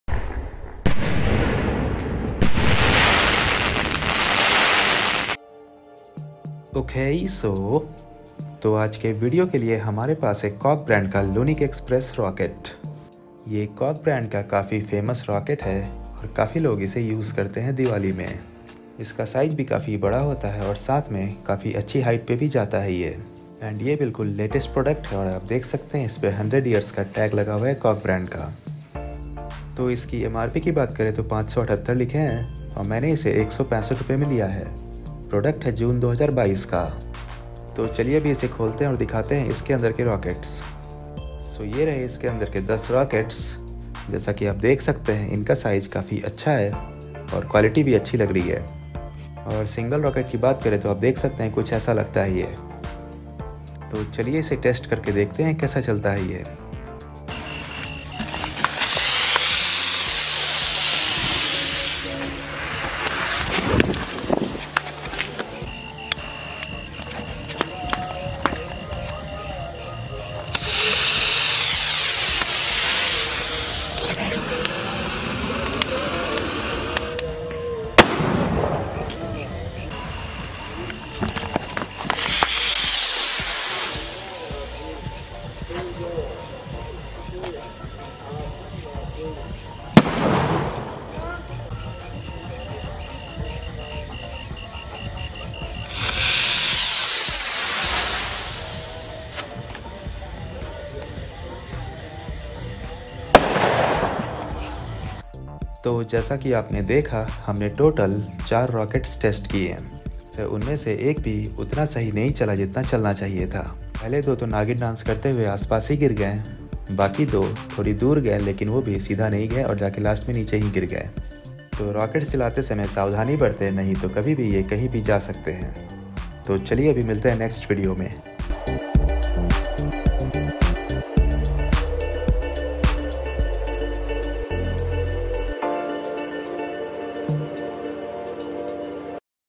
LUNIK EXPRESS Rocket 2022 from CockBrand - Diwali Murga Chaap Patake
So, today we are testing Lunik Express Rocket from Cockbrand Sri kaliswari fireworks. It price is Rs.165 in wholesale and is a good rocket for Diwali 2022.

My Gear -
iPhone 12 Mini
Canon 200D DSLR
Boya Mic
Simpex Tripod